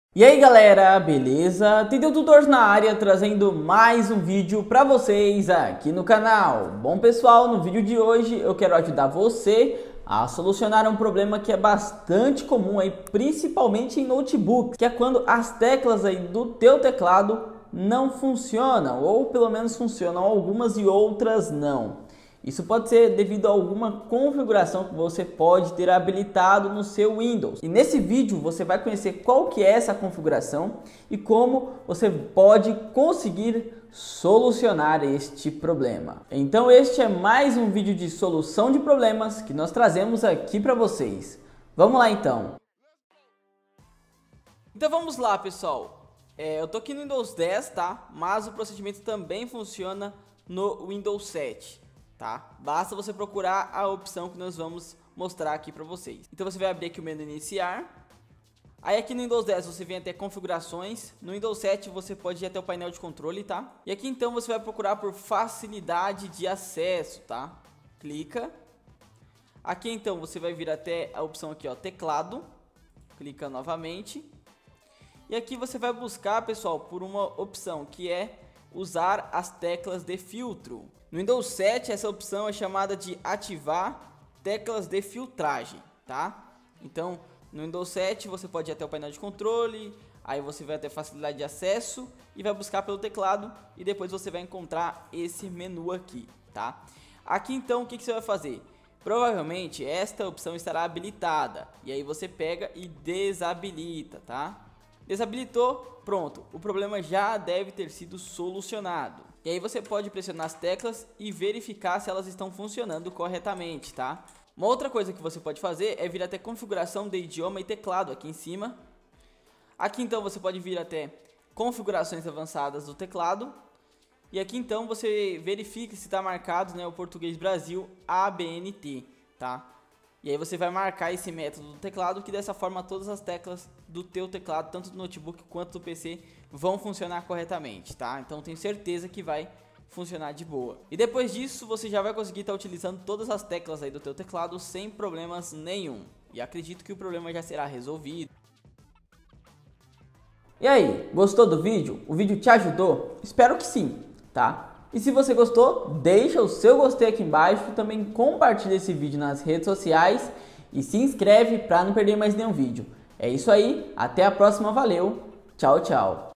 Teclado não funciona todas as teclas? APRENDA RESOLVER!
Neste vídeo você vai aprender a resolver um problema bastante comum no teclado do seu computador ou notebook, trata-se de um erro onde algumas teclas do seu teclado não funciona, confira a solução em mais um vídeo!